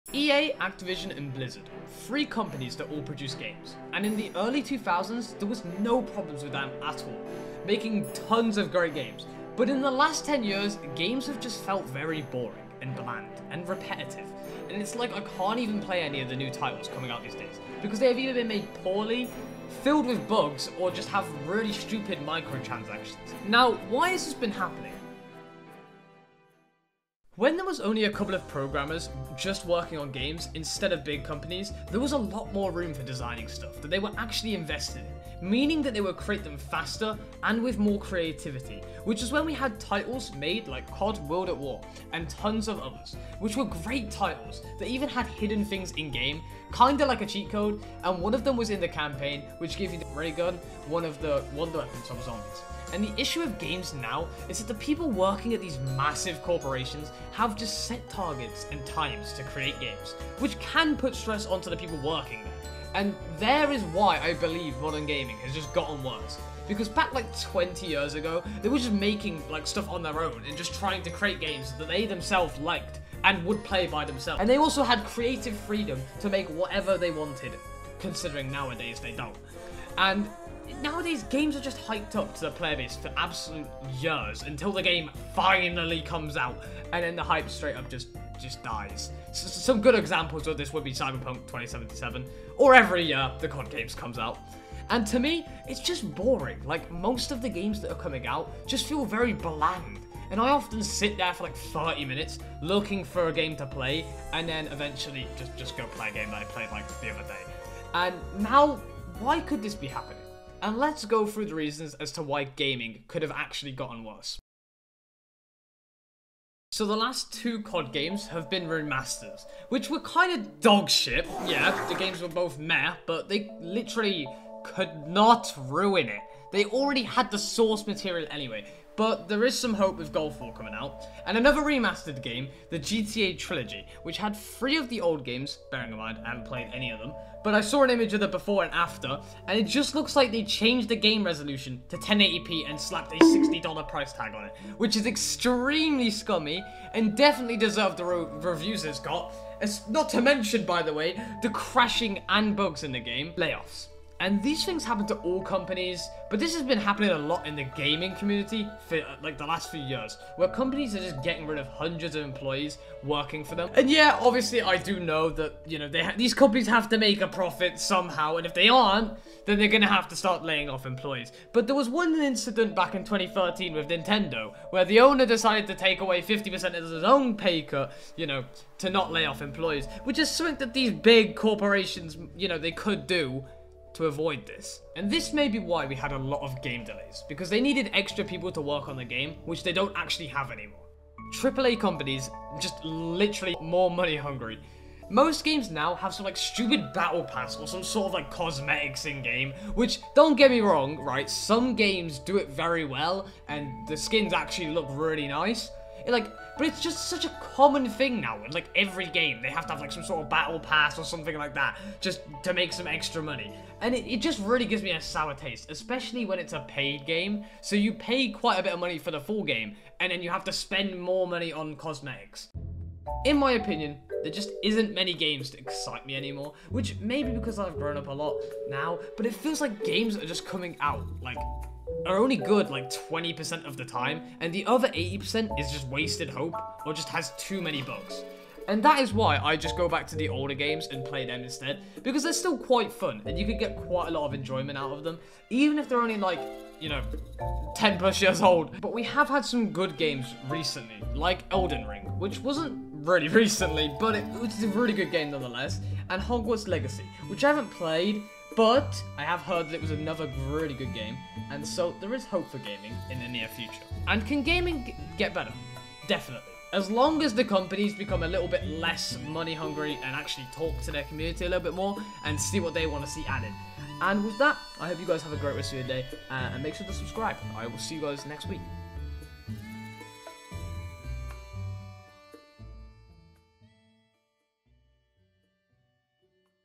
What Happened To Modern Gaming?
Uninspired and Bug Filled Mess.

Modern Gaming is crap with layoffs games being awful and filled to the brim with bugs and everything in between. It just feels like gaming feels extremely boring right now and in this video I explain the reasons and what is happening right now.